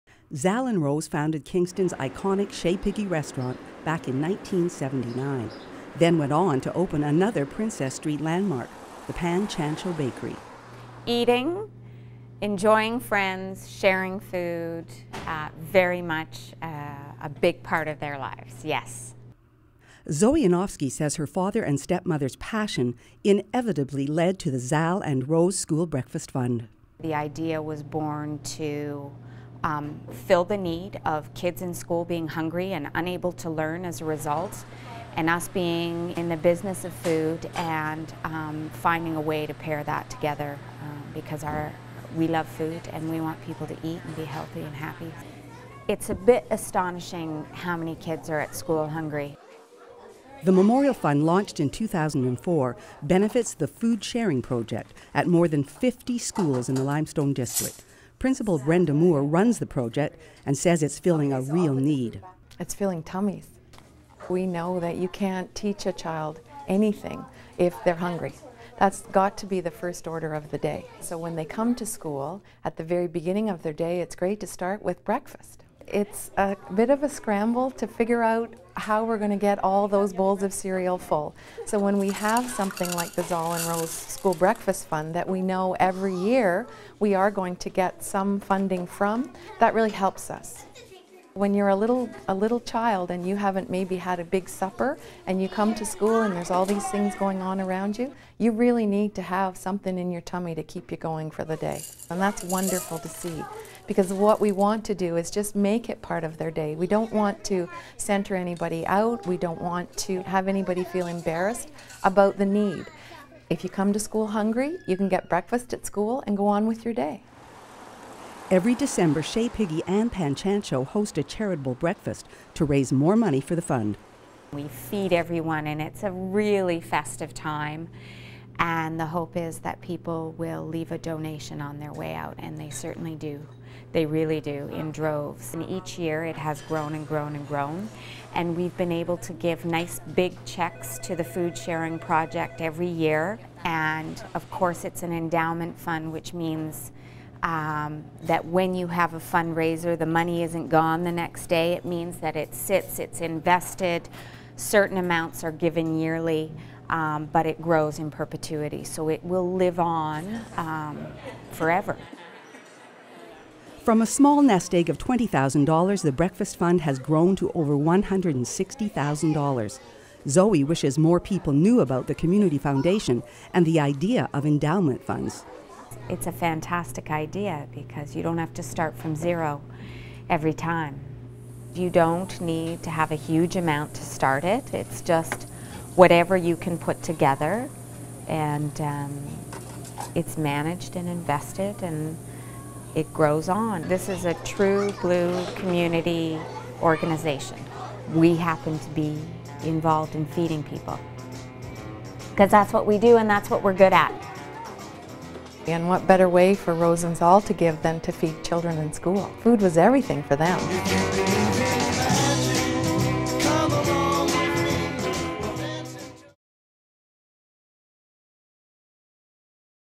Zal and Rose Yanovsky Breakfast Fund
The origins of the Zal and Rose Yanovsky Breakfast fund in Kingston and the yearly breakfasts at Chez Piggy and Pan Chancho, two of Kingston's best known restaurants.